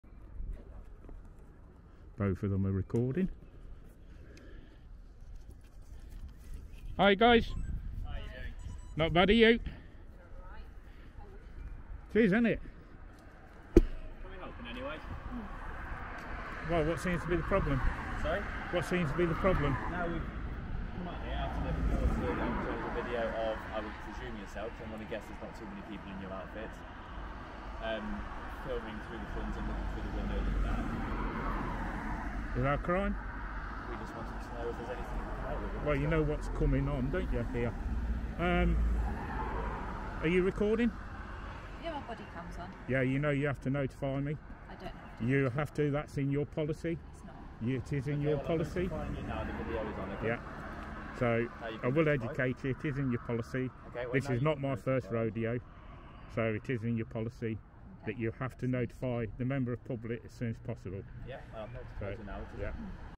UK Law - Police Deny they have to inform you when they are filming on Body Cam- -Quorn Country Hotel
Loughborough Refuse to admit they have to inform a member of the Public when they are recording on BodyCam.
In accordance with UK law, it is essential for Loughborough (or any organization) to inform a member of the public when they are being recorded on a BodyCam or any other surveillance device. The Data Protection Act 2018 and the General Data Protection Regulation (GDPR) require transparency and fair processing of personal data.

Failing to inform individuals about recording their activities on BodyCams can be considered a breach of privacy rights. Organizations must provide clear notices to the public, explaining the purpose of recording, the legal basis for processing the data, and how long the data will be retained.

If Loughborough refuses to acknowledge its legal obligation to inform the public about recording on BodyCams, it is important for concerned individuals to assert their rights and seek clarification from the appropriate authorities or data protection regulatory bodies.

Remember, open and honest communication about data processing is fundamental to safeguarding individuals' privacy and complying with the law.

👍 PLEASE TAP THE LIKE BUTTON LETS LET GET THIS VIDEO TO 1000 LIKES 🥰

The Buzz does not own the rights to these videos and pictures.